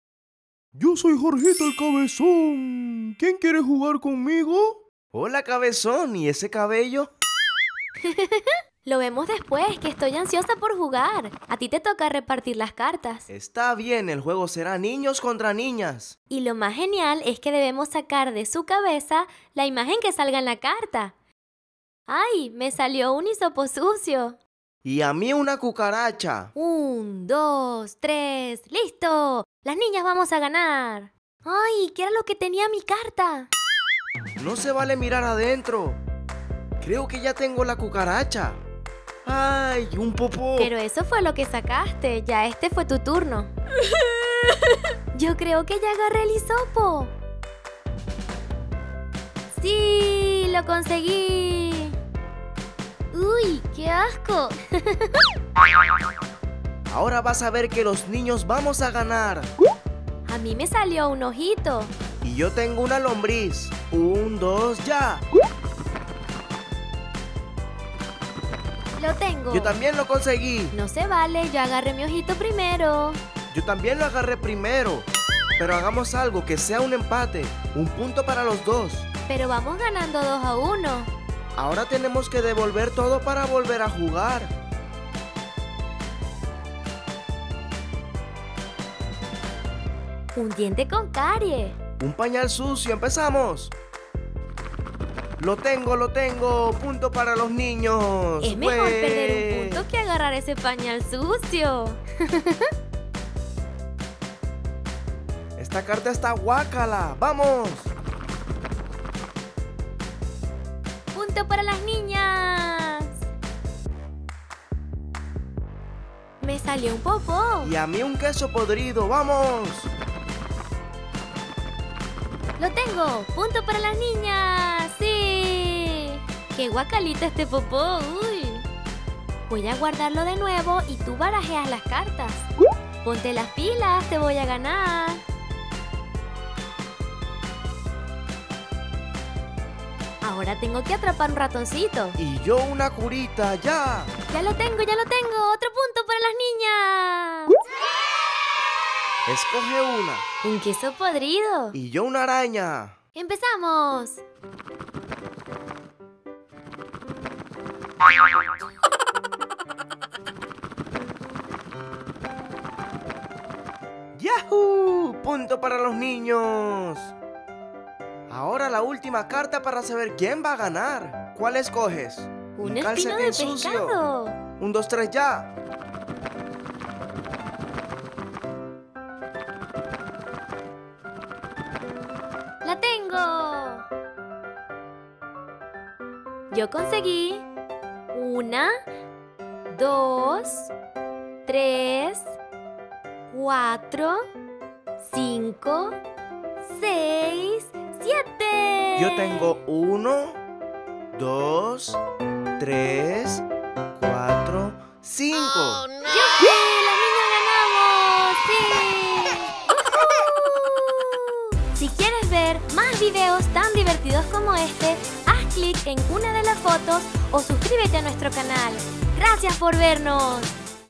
TotoyKids Jugando con Jorgito la Cabeza Loca Niños contra Niñas!!! Quién ganará esta vez???
En este divertido video vemos a TotoyKids jugando con Jorgito, la Cabeza Loca niños contra niñas!!! Quién ganará esta vez??? Vamos a sacar divertidos objetos de la cabeza de nuestro amigo Jorgito!!! TotoyKids. Muy divertido :)